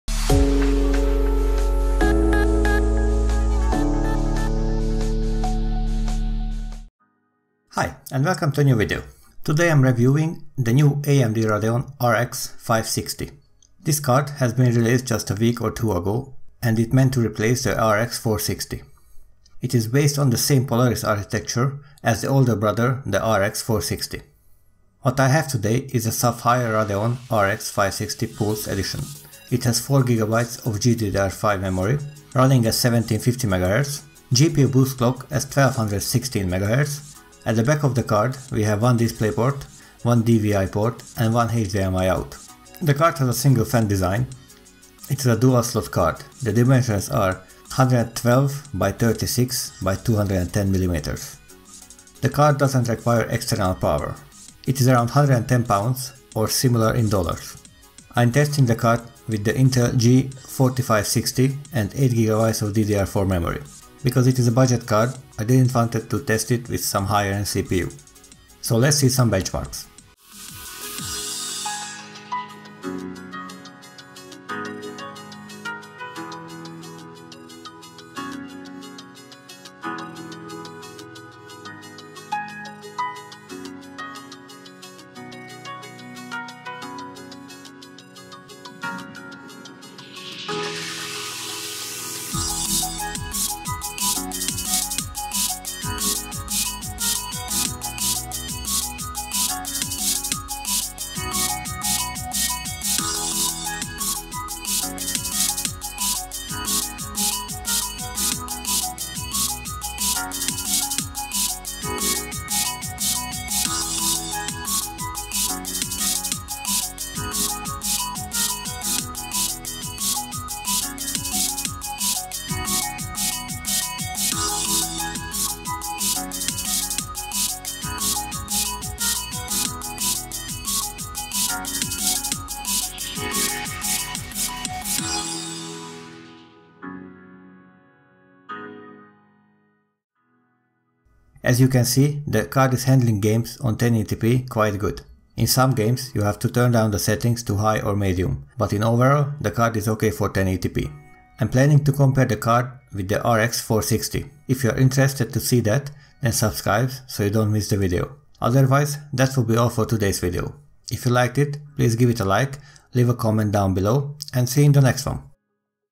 AMD Radeon RX 560
Today I’m testing the AMD Radeon RX 560 from Sapphire.

Follow on

Amazon UK
Amazon US